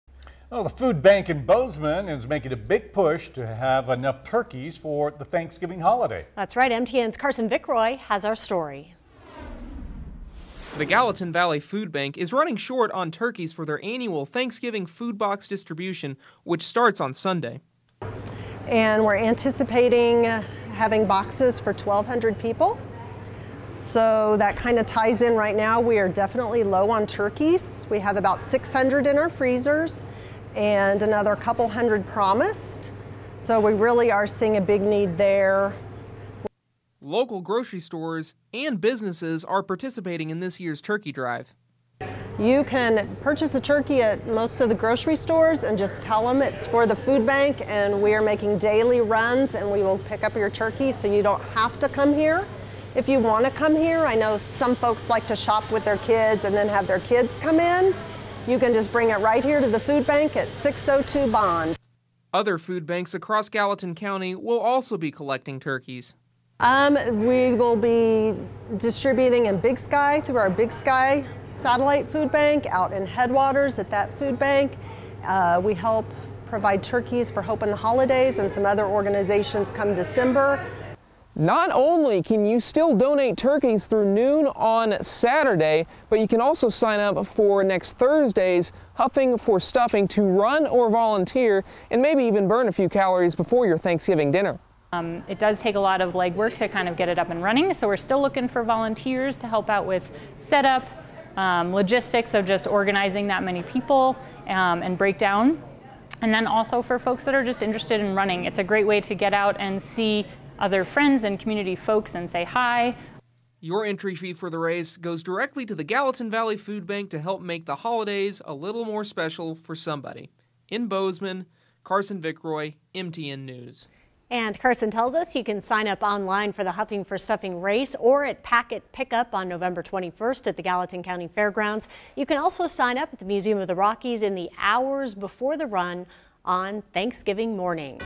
Gobble, gobble? Gallatin Valley Food Bank low on turkey donations
BOZEMAN, Mont. - The local food bank in Bozeman is making a big push to have enough turkeys for families across the county.